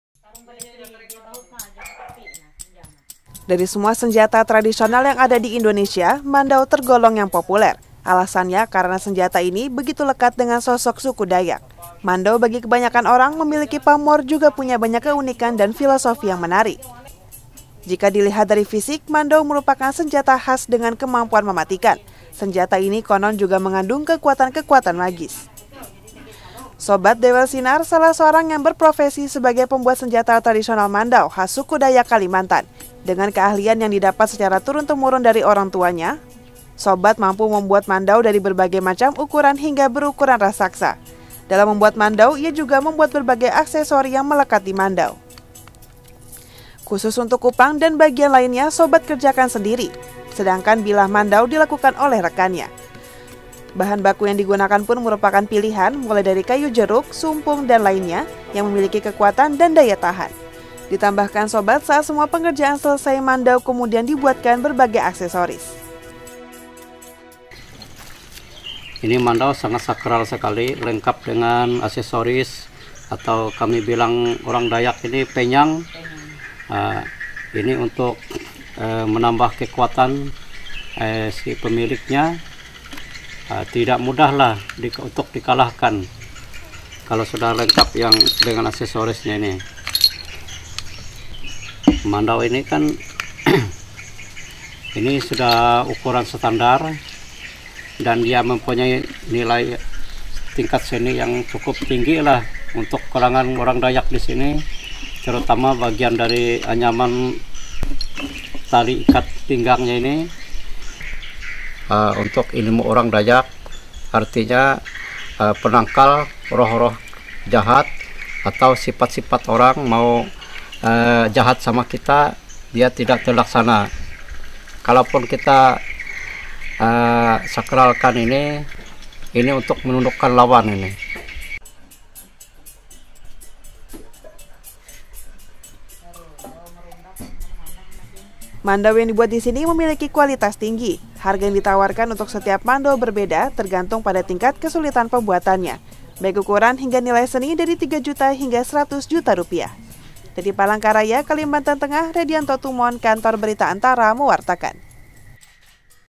Mengenal mandau kalimantan, dan jenis-jenis mandau
Mengenal jenis mandau kalimantan yang penuh filosofi dan simbolis pada sebilah mandau,